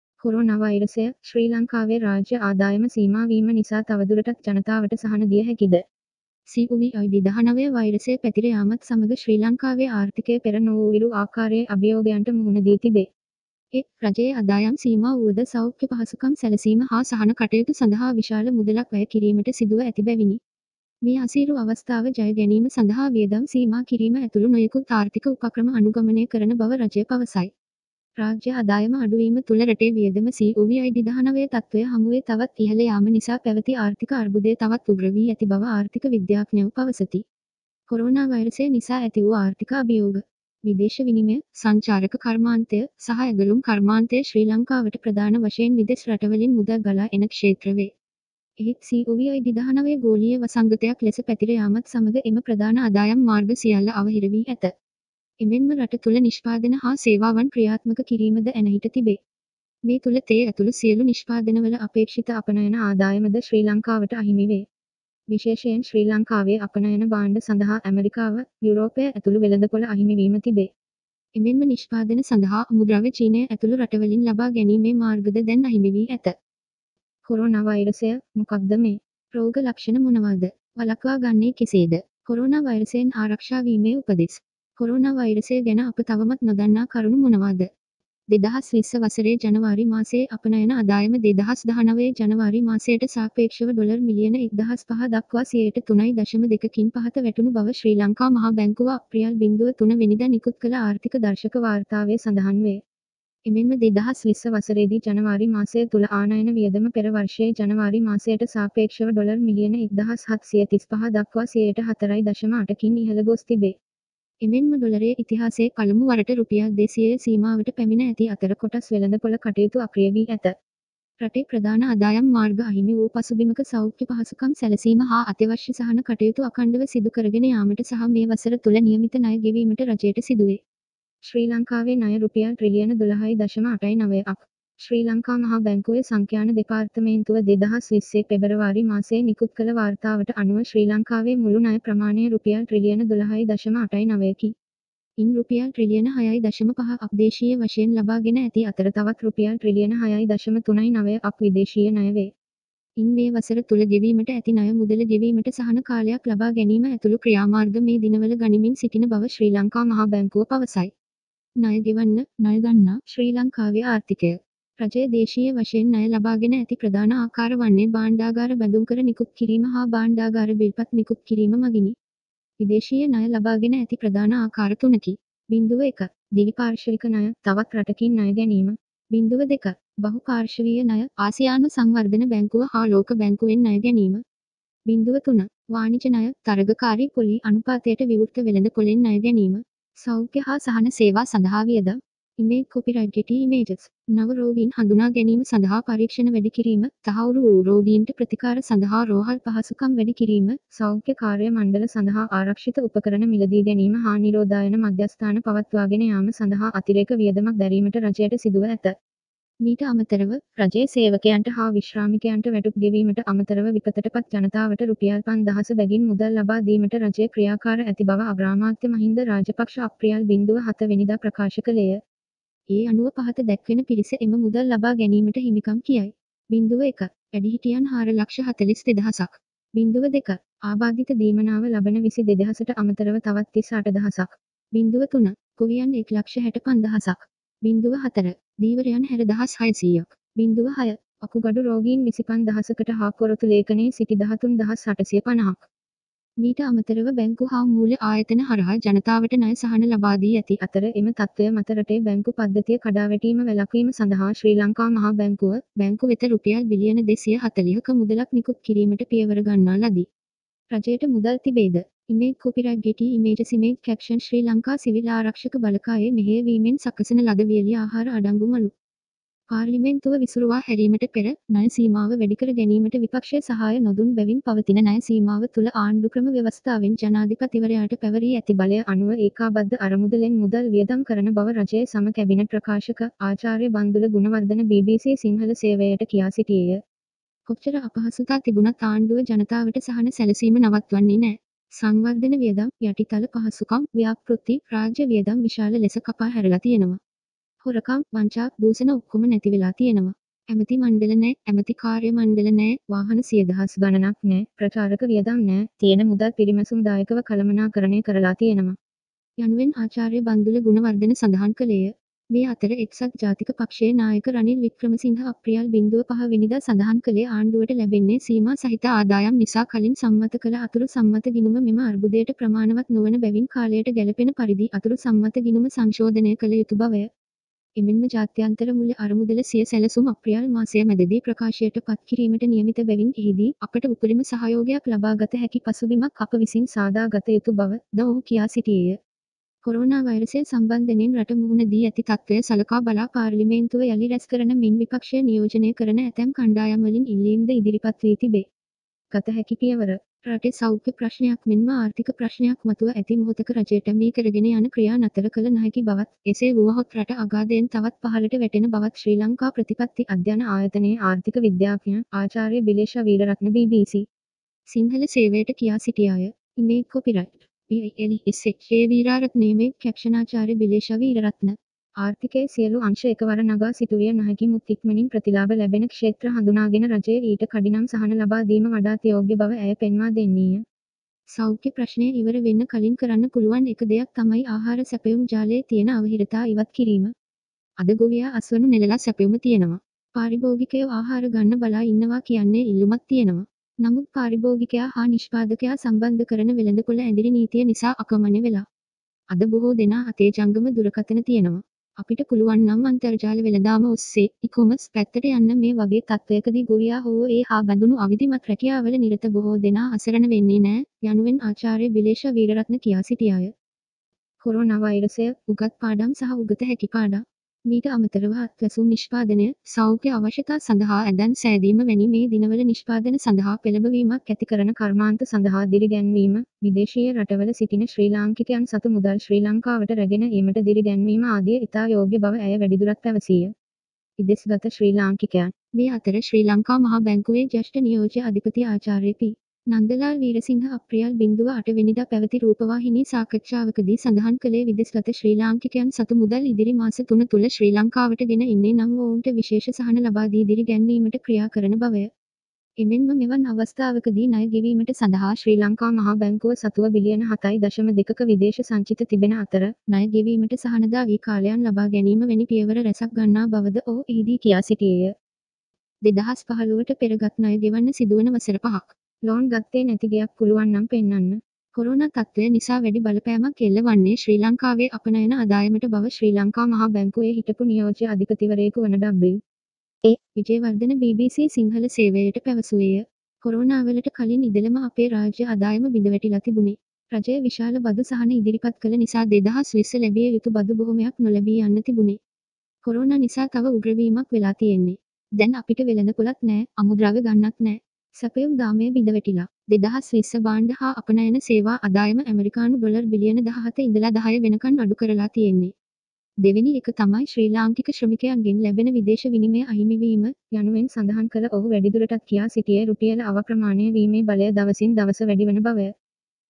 කොරෝනා වෛරසය: ශ්‍රී ලංකාවේ රාජ්‍ය ආදායම සීමා වීම නිසා තවදුරටත් ජනතාවට සහන දිය හැකි ද? COVID-19 ව
කොරෝනා වෛරසය: ශ්‍රී ලංකාවේ රාජ්‍ය ආදායම සීමා වීම නිසා තවදුරටත් ජනතාවට සහන දිය හැකි ද?

COVID-19 වෛරසය පැතිර යාමත් සමග ශ්‍රී ලංකාවේ ආර්ථිකය පෙර නොවූවිරූ ආකාරයේ අභියෝගයන්ට මුහුණ දී තිබේ. ඒ, රජයේ අදායම් සීමා වුව ද සෞඛ්‍ය පහසුකම් සැලසීම හා සහන කටයුතු සඳහා විශාල මුදලක් වැය කිරීමට සිදුව ඇති බැවිනි.

මේ අසීරු අවස්ථාව ජය ගැනීම සඳහා වියදම් සීමා කිරීම ඇතුළු නොයෙකුත් ආර්ථික උපක්‍රම අනුගමනය කරන බව රජය පවසයි. රාජ්‍ය අදායම අඩුවීම තුළ රටේ වියදම COVID-19 තත්ත්වය හමුවේ තවත් ඉහළ යාම නිසා පැවති ආර්ථික අර්බුදය තවත් උග්‍ර වී ඇති බව ආර්ථික විද්‍යාඥයෝ පවසති.

කොරෝනාවෛරසය නිසා ඇතිවූ ආර්ථික අභියෝග

විදේශ විනිමය, සංචාරක කර්මාන්තය, සහ ඇඟලුම් කර්මාන්තය ශ්‍රී ලංකාවට ප්‍රධාන වශයෙන් විදෙස් රටවලින් මුදල් ගලා එන ක්ෂේත්‍ර වේ. එහෙත් COVID-19 ගෝලීය වසංගතයක් ලෙස පැතිර යාමත් සමග එම ප්‍රධාන අදායම් මාර්ග සියල්ල අවහිර වී ඇත.

එමෙන් ම රට තුළ නිෂ්පාදන හා සේවාවන් ක්‍රියාත්මක කිරීම ද ඇණහිට තිබේ. මේ තුළ තේ ඇතුළු සියලු නිෂ්පාදනවල අපේක්ෂිත අපනයන ආදායම ද ශ්‍රී ලංකාවට අහිමි වේ. විශේෂයෙන් ශ්‍රී ලංකාවේ අපනයන භාණ්ඩ සඳහා ඇමරිකාව, යුරෝපය ඇතුළු වෙළඳපොල අහිමිවීම තිබේ. එමෙන්ම නිෂ්පාදන සඳහා අමුද්‍රව්‍ය චීනය ඇතුළු රටවලින් ලබා ගැනීමේ මාර්ග ද දැන් අහිමිවී ඇත.

කොරෝනා වෛරසය: මොකක් ද මේ? රෝග ලක්ෂණ මොනවා ද? වළක්වා ගන්නේ කෙසේ ද?

කොරෝනා වෛරසයෙන් ආරක්ෂා වීමේ උපදෙස්

කොරෝනා වෛරසය ගැන අප තවමත් නොදන්නා කරුණු මොනවා ද?

2020 වසරේ ජනවාරි මාසයේ අපනයන අදායම 2019 ජනවාරි මාසයට සාපේක්ෂව ඩොලර් මිලියන 1005 දක්වා 3.2%කින් පහත වැටුණු බව ශ්‍රී ලංකා මහ බැංකුව අප්‍රියල් 03 වෙනිදා නිකුත් කළ ආර්ථික දර්ශක වාර්තාවේ සඳහන් වේ. එමෙන්ම 2020 වසරේදී ජනවාරි මාසය තුළ ආනයන වියදම පෙර වර්ෂයේ ජනවාරි මාසයට සාපේක්ෂව ඩොලර් මිලියන 1735 දක්වා 4.8%කින් ඉහළ ගොස් තිබේ.

එමෙන් ම ඩොලරය ඉතිහාසයේ පළමු වරට රුපියල් 200.00 සීමාවට පැමිණ ඇති අතර කොටස් වෙළඳපොළ කටයුතු අක්‍රිය වී ඇත.

රටේ ප්‍රධාන අදායම් මාර්ග අහිමි වූ පසුබිමක සෞඛ්‍ය පහසුකම් සැලසීම හා අත්‍යවශ්‍ය සහන කටයුතු අඛණ්ඩව සිදු කරගෙන යාමට සහ මේ වසර තුළ නියමිත ණය ගෙවීමට රජයට සිදුවේ.

ශ්‍රී ලංකාවේ ණය රුපියල් ට්‍රිලියන 12.89ක්

ශ්‍රී ලංකා මහ බැංකුවේ සංඛ්‍යාන දෙපාර්තමේන්තුව 2020 පෙබරවාරි මාසයේ නිකුත් කළ වාර්තාවට අනුව ශ්‍රී ලංකාවේ මුළු ණය ප්‍රමාණය රුපියල් ට්‍රිලියන 12.89කි. ඉන් රුපියල් ට්‍රිලියන 6.5ක් දේශීය වශයෙන් ලබා ගෙන ඇති අතර තවත් රුපියල් ට්‍රිලියන 6.39ක් විදේශීය ණය වේ. ඉන් මේ වසර තුළ ගෙවීමට ඇති ණය මුදල ගෙවීමට සහන කාලයක් ලබා ගැනීම ඇතුළු ක්‍රියාමාර්ග මේ දිනවල ගනිමින් සිටින බව ශ්‍රී ලංකා මහ බැංකුව පවසයි.

ණය ගෙවන්න 'ණය ගන්නා' ශ්‍රී ලංකාවේ ආර්ථිකය

රජය දේශීය වශයෙන් ණය ලබාගෙන ඇති ප්‍රධාන ආකාර වන්නේ භාණ්ඩාගාර බැඳුම්කර නිකුත් කිරීම හා භාණ්ඩාගාර බිල්පත් නිකුත් කිරීම මගිනි. විදේශීය ණය ලබාගෙන ඇති ප්‍රධාන ආකාර තුනකි.

01. දිවිපාර්ශවික ණය - [තවත් රටකින් ණය ගැනීම ]

02. බහු පාර්ශවීය ණය - [ආසියානු සංවර්ධන බැංකුව හා ලෝක බැංකුවෙන් ණය ගැනීම]

03. වාණිජ ණය - [තරගකාරී පොළී අනුපාතයට විවෘත වෙළඳ පොළෙන් ණය ගැනීම]

සෞඛ්‍ය හා සහන සේවා සඳහා වියදම්

￼Image copyrightGETTY IMAGES

නව රෝගීන් හඳුනා ගැනීම සඳහා පරීක්ෂණ වැඩි කිරීම, තහවුරු වූ රෝගීන්ට ප්‍රතිකාර සඳහා රෝහල් පහසුකම් වැඩි කිරීම, සෞඛ්‍ය කාර්ය මණ්ඩල සඳහා ආරක්‍ෂිත උපකරණ මිලදීගැනීම හා නිරෝධායන මධ්‍යස්ථාන පවත්වාගෙන යාම සඳහා අතිරේක වියදමක් දැරීමට රජයට සිදුව ඇත.

මීට අමතරව, රජයේ සේවකයන්ට හා විශ්‍රාමිකයන්ට වැටුප් ගෙවීමට අමතරව විපතටපත් ජනතාවට රුපියල් 5000.00 බැගින් මුදල් ලබා දීමට රජය ක්‍රියාකාර ඇති බව අග්‍රාමාත්‍ය මහින්ද රාජපක්ෂ අප්‍රියල් 07 වෙනිදා ප්‍රකාශ කළේය. ඒ අනුව පහත දැක්වෙන පිරිස එම මුදල් ලබා ගැනීමට හිමිකම් කියයි.

01. වැඩිහිටියන් හාර ලක්ෂ හතලිස් දෙදහසක්

02. ආබාධිත දීමනාව ලබන විසි දෙදෙහසට අමතරව තවත් තිස් අටදහසක්

03. ගොවියන් එක් ලක්ෂ හැට පන්දහසක්

04. ධීවරයන් හැර දහස් හයසීයක්

06. වකුගඩු රෝගීන් විසිපන් දහසකට හා පොරොතු ලේඛනයේ සිටි දහතුන් දහස් අටසිය පනහක්

මීට අමතරව බැංකු හා මූල්‍ය ආයතන හරහා ජනතාවට ණය සහන ලබා දී ඇති අතර එම තත්ත්වය මත රටේ බැංකු පද්ධතිය කඩාවැටීම වැළක්වීම සඳහා ශ්‍රී ලංකා මහ බැංකුව, බැංකු වෙත රුපියල් බිලියන 240ක මුදලක් නිකුත් කිරීමට පියවර ගන්නා ලදී.

රජයට මුදල් තිබේ ද?

￼Image copyrightGETTY IMAGESImage captionශ්‍රී ලංකා සිවිල් ආරක්ෂක බලකායේ මෙහෙය වීමෙන් සකසන ලද වියලි ආහාර අඩංගු මලු

පාර්ලිමේන්තුව විසුරුවා හැරීමට පෙර, ණය සීමාව වැඩිකර ගැනීමට විපක්ෂය සහාය නොදුන් බැවින් පවතින ණය සීමාව තුළ ආණ්ඩුක්‍රම ව්‍යවස්ථාවෙන් ජනාධිපතිවරයාට පැවරී ඇති බලය අනුව ඒකාබද්ධ අරමුදලෙන් මුදල් වියදම් කරන බව රජයේ සම කැබිනට් ප්‍රකාශක, ආචාර්ය බන්දුල ගුණවර්ධන බීබීසී සිංහල සේවයයට කියා සිටියේය.

''කොච්චර අපහසුතා තිබුණත් ආණ්ඩුව ජනතාවට සහන සැලසීම නවත්වන්නේ නෑ. සංවර්ධන වියදම්, යටිතල පහසුකම්, ව්‍යාපෘති, රාජ්‍ය වියදම් විශාල ලෙස කපා හැරලා තියෙනවා. හොරකම්, වංචා, දුෂණ ඔක්කොම නැතිවෙලා තියෙනවා. ඇමති මණ්ඩල නෑ, ඇමති කාර්ය මණ්ඩල නෑ, වාහන සියදහස් ගණනක් නෑ, ප්‍රචාරක වියදම් නෑ, තියෙන මුදල් පිරිමැසුම්දායකව කළමනාකරණය කරලා තියෙනවා.'' යනුවෙන් ආචාර්ය බන්දුල ගුණවර්ධන සඳහන් කළේය.

මේ අතර එක්සත් ජාතික පක්ෂයේ නායක රනිල් වික්‍රමසිංහ අප්‍රියල් 05 වෙනිදා සඳහන් කළේ ආණ්ඩුවට ලැබෙන්නේ සීමා සහිත ආදායම් නිසා කලින් සම්මත කළ අතුරු සම්මත ගිණුම මෙම අර්බුදයට ප්‍රමාණවත් නොවන බැවින් කාලයට ගැලපෙන පරිදි අතුරු සම්මත ගිණුම සංශෝදනය කළ යුතු බවය.

එමෙන්ම ජාත්‍යන්තර මුල්‍ය අරමුදල සිය සැළසුම් අප්‍රියල් මාසය මැදදී ප්‍රකාශයට පත් කිරීමට නියමිත බැවින් එහිදී "අපට උපරිම සහයෝගයක් ලබාගත හැකි පසුබිමක් අප විසින් සාදා ගත යුතු බව" ද ඔහු කියා සිටියේය.

කොරෝනාවෛරසය සම්බන්ධන්යෙන් රට මුහුණ දී ඇති තත්ත්වය සලකා බලා පාර්ලිමේ